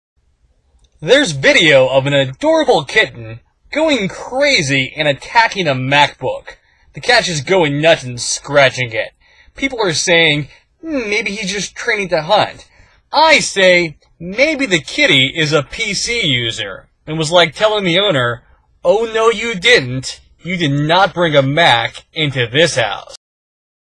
Adorable kitten freaks out, attacks MacBook
omebody get this kitten a mouse! At least the little fella would know what to do with it, instead of being f-r-e-a-k-e-d out by the monitor on this MacBook. The curious kitty grasps for the icons on the screen with two paws while unwittingly moving them around with the others, and the result is pure awwwwwwwwww. Eventually the cutie activates some techno music, checks behind the screen and plays around with the power cord before becoming distracted by something else that's more interesting and bounding off, all nine adorable lives intact.